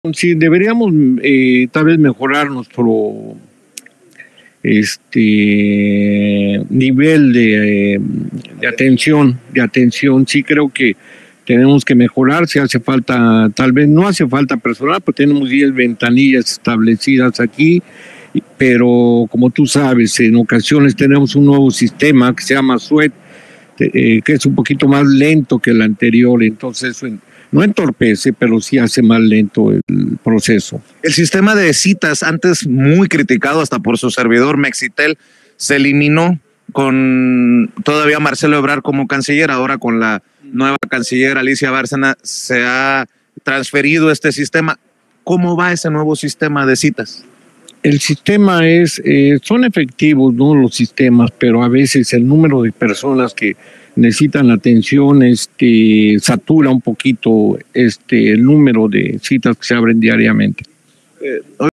RECONOCE CÓNSUL QUE SÍ DEBERÍAN MEJORAR NIVEL DE ATENCIÓN EN CUANTO A TIEMPOS DE ESPERA
Ante las quejas por los tiempos de espera, el cónsul de Protección en el Consulado General de México en Santa Ana, California Alejandro Mendoza reconoció que sí deberían sí mejorar su nivel de atención, no hace falta personal, pero hay un nuevo sistema que es un poco más lento que el anterior